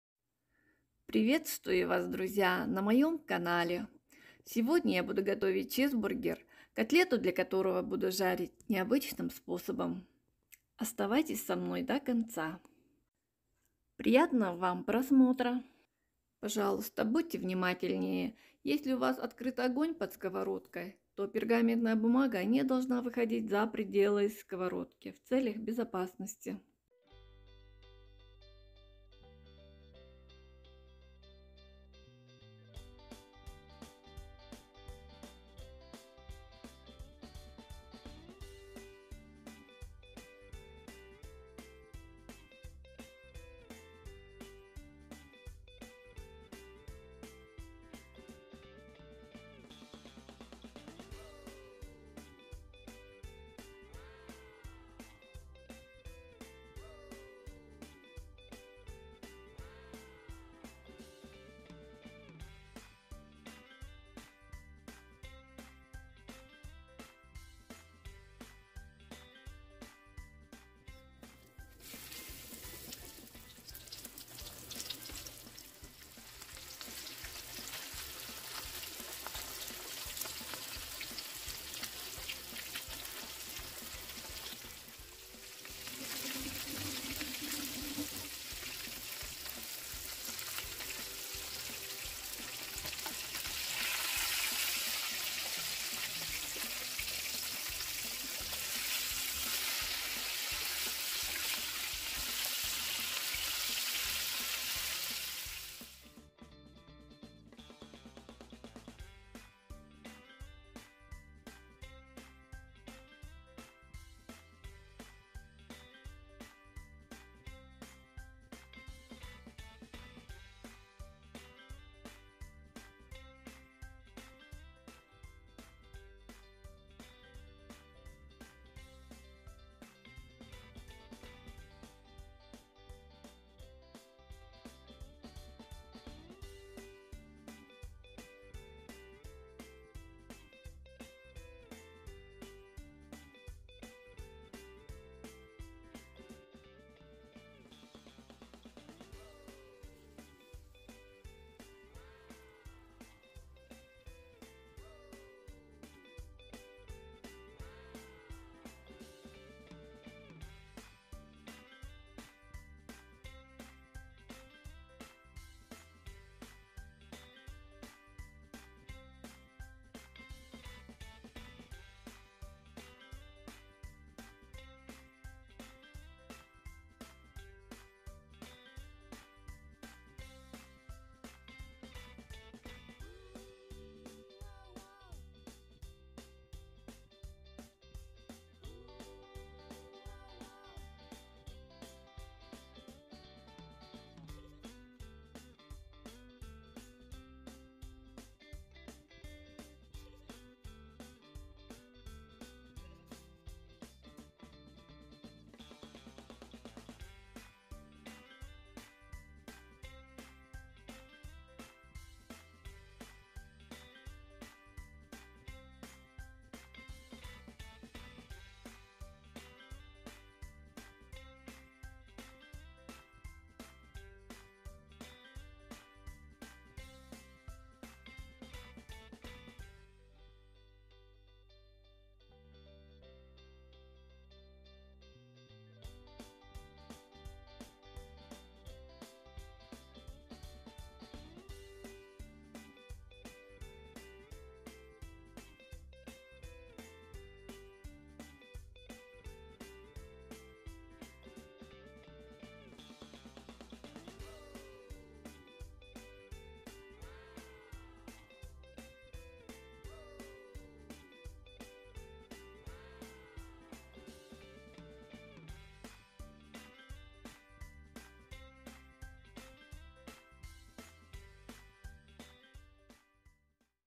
Как Пожарить Котлету не Запачкав Плиту и Сковороду? How fried cutlets without staining the stove..?
Добро пожаловать друзья на канал Dish Resipes!  Благодарна вам, что смотрите мои ролики! 💞 Оцените сегодняшний лайфхак!

Предлагаю вам использовать пергаментную бумагу не только для духовки, но и в готовке сковороде. Это очень удобно!
Экономит время и силы!

Все сталкивались ситуацией, когда жарим на сковороде мясо, котлеты, рыбу и т.п. масло любит разбрызгиваться вокруг.🙁
Пергамент помогает избежать этого.
А если сделать крышечку из пергамента, то конденсат не будет накапливаться и брызгаться. Необходимое тепло сохранится и приготовится наше блюдо без лишних хлопот!
 ❗Внимание! Только сохраняйте технику безопасности! Если открытый огонь под сковородой, пергамент не должен выходит за пределы сковороды во избежании возгорания!

У меня были замороженные котлеты из говядины, я их разморозила и жарила 12 мин. В начале минуты две огонь выше среднего, потом снизила на среднее. После уменьшила огонь и под крышкой из пергамента протушила 7 мин. Если котлеты из курицы, времени уйдет меньше.

И вуаля!  Чизбургер своими руками готов!
 Готовьте с любовью и у вас все получиться!

Welcome friends to the Dish Resipes channel!  💞 Check out today's hack!

 I suggest you use parchment paper not only for the oven, but also in the cooking pan.  It is very convenient!
 Saves time and effort!

 Everyone faced a situation when we fry meat, meatballs, fish, etc. in a pan.  oil loves to splatter around.🙁
 Parchment helps to avoid this.
 And if you make a lid of parchment, then the condensate will not accumulate and splash.  The necessary heat will be preserved and our dish will be prepared without any hassle!
  ❗Attention!  Just keep safety precautions!  If an open fire is under the pan, the parchment must not go out of the pan to avoid fire!

 I had frozen cutlets beef, I thawed them and fried them for 12 minutes.  At the beginning of the minute, two fires were above average, then lowered to medium.  Then she reduced the heat and simmered for 7 minutes under a parchment lid. If chicken cutlets, it will take less time.
 And voila!  Your own cheeseburger is ready!
  Cook with love and you will succeed!